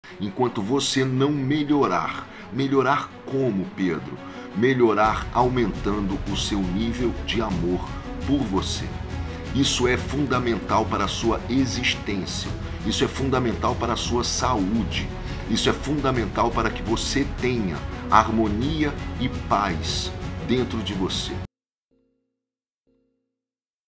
A importância de cultivar o amor-próprio para sua existência.
Você precisa melhorar tendo mais amor-próprio!

Dê um Ponto de Virada na história do filme da sua vida!